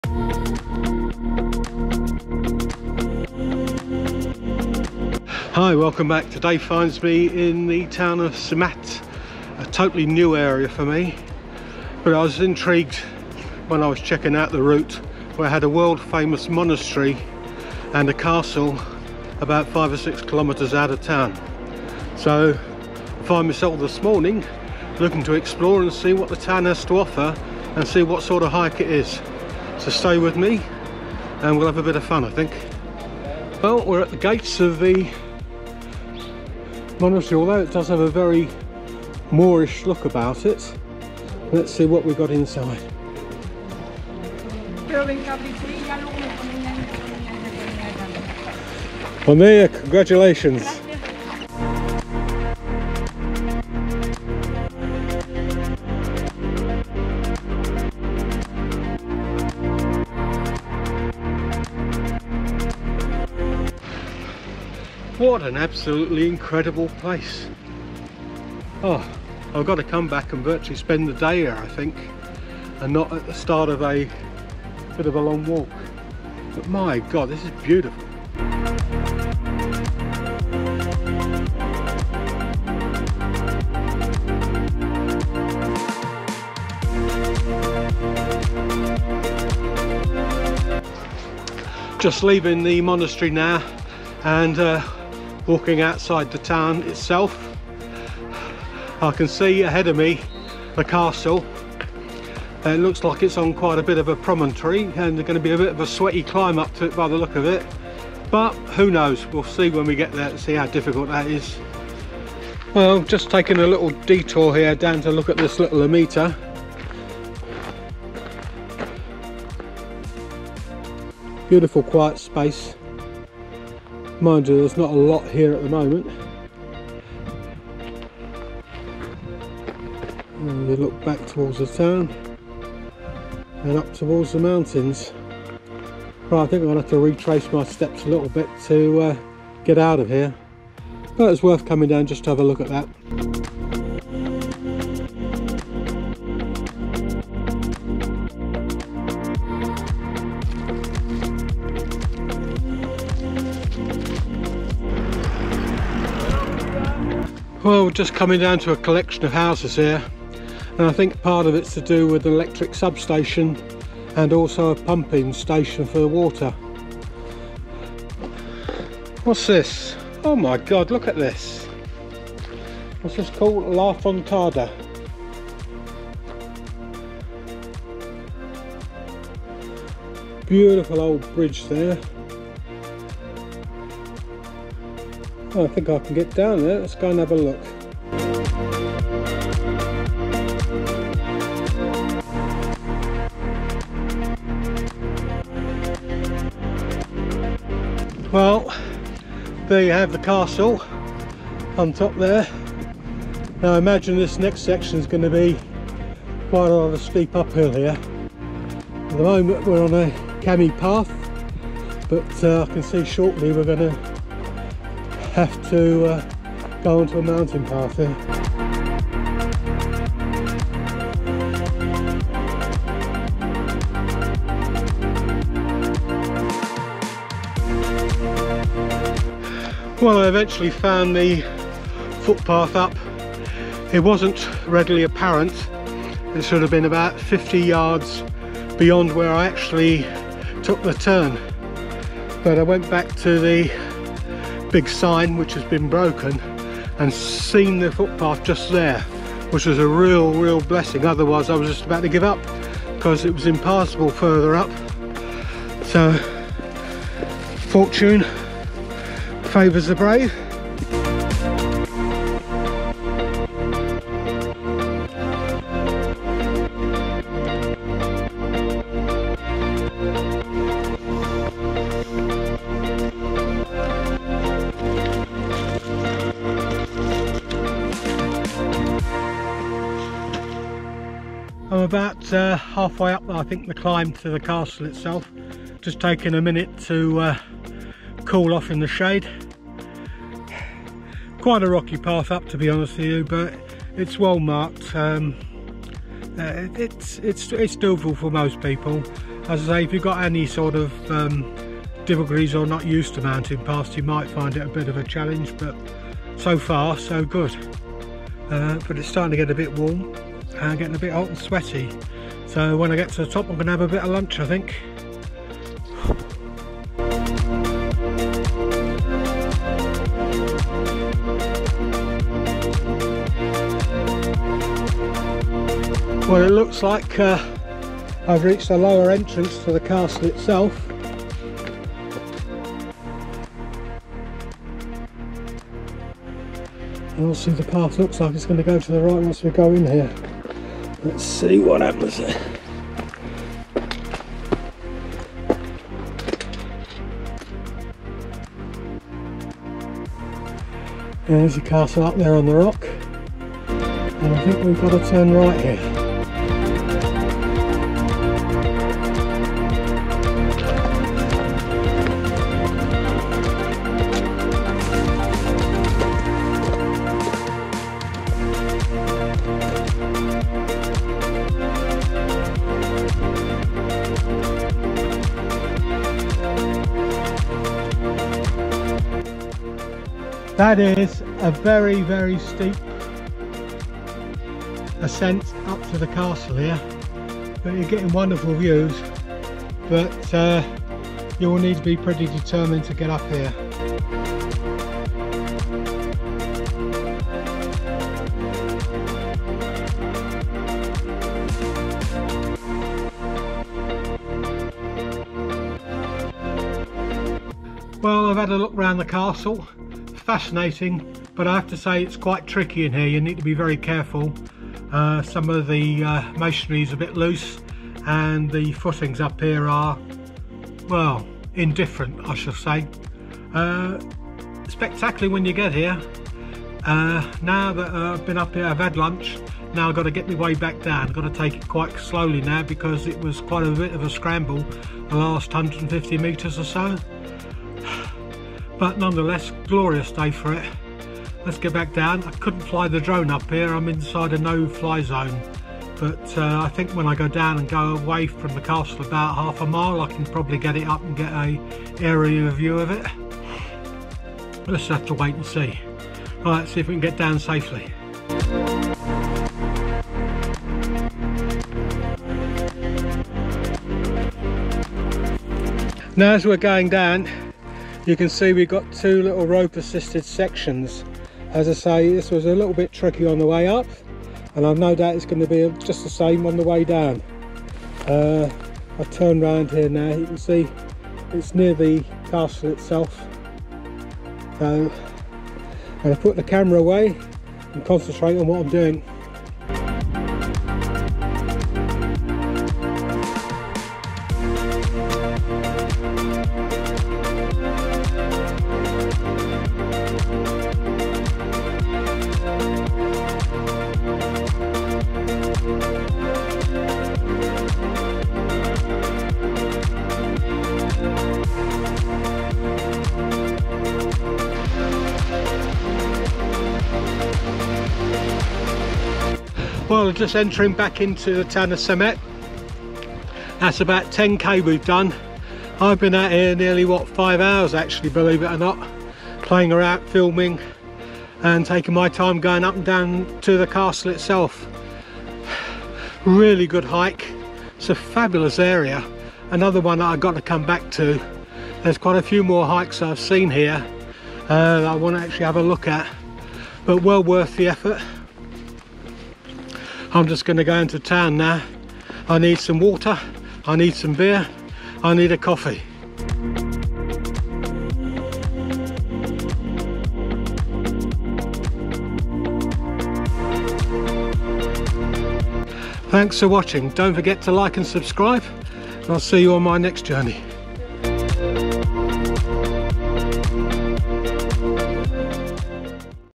Epic 10k Hike: Monastery of Santa Maria de Valldigna to Castell Marinyen | Valencia, Spain
🥾 Lace up your boots and join us on an unforgettable 10-kilometer adventure through the breathtaking landscapes of Valencia's Simat de la Valldigna region! This exhilarating hike takes you from the awe-inspiring Monastery of Santa Maria de Valldigna to the majestic heights of Castell Marinyen.

 Begin your journey immersed in centuries of history at the Monastery of Santa Maria de Valldigna, a stunning example of ancient architecture nestled in the heart of nature. Discover the secrets of this ancient spiritual sanctuary before embarking on a trail that promises both challenge and reward.

As you ascend, be prepared for a steep climb that will test your mettle and reward you with panoramic views that stretch as far as the eye can see. The rugged terrain offers a thrilling challenge for hikers of all levels, and every step brings you closer to the awe-inspiring Castell Marinyen.

 At the pinnacle of your journey, Castell Marinyen stands tall, offering an unparalleled vantage point to soak in the beauty of Valencia's countryside. Take a moment to catch your breath and marvel at the picturesque vistas that unfold before you.

Don't forget to capture these unforgettable moments and share your adventure using ValenciaHike! This 10k trek is an experience you won't want to forget, and we can't wait for you to join us on this incredible journey.

Remember, the world is waiting for you to discover it, one step at a time!